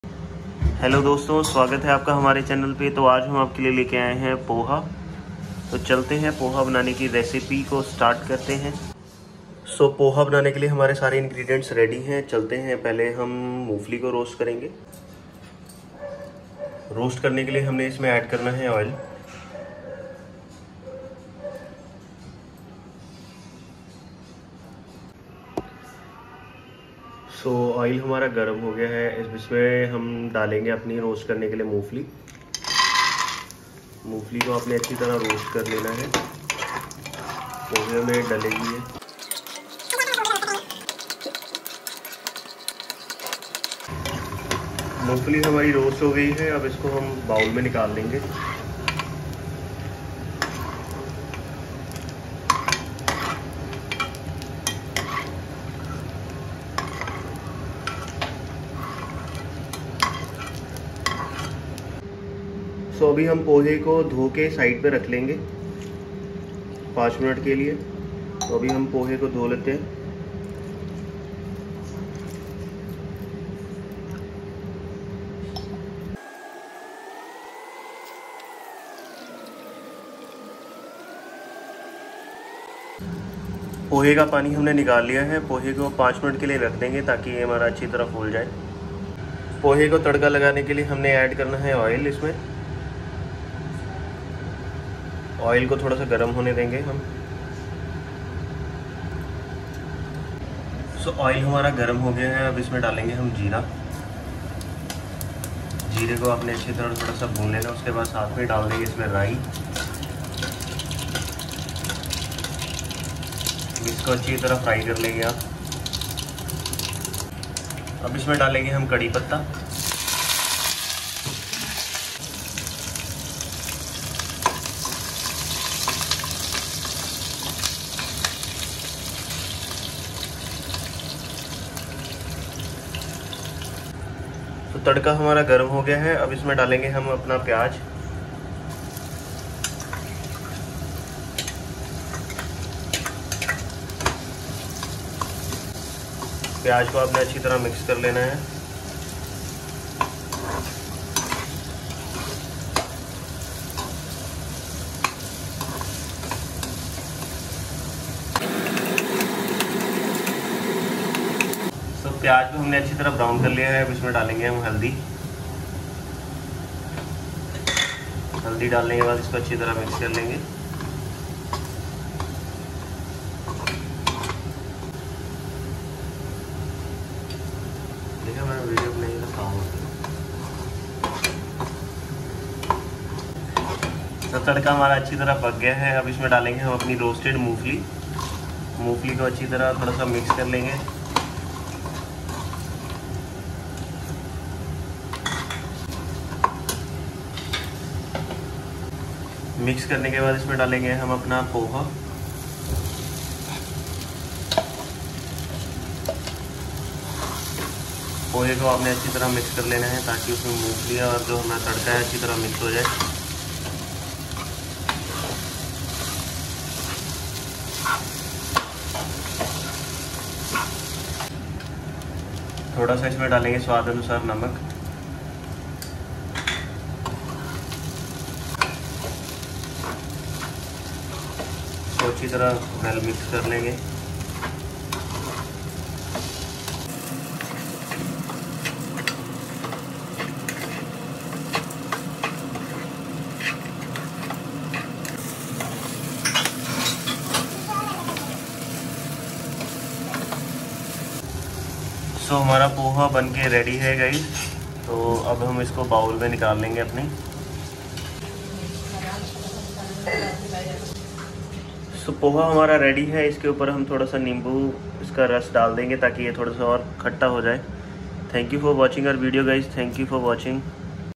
पोहा नाश्ते की रेसिपी | पोहा मॉर्निंग ब्रेकफास्ट रेसिपी | Healthy morning breakfast
सुबह का नाश्ता बनाएं स्वास्थ्यवर्धक और आसान नाश्ता बनाओ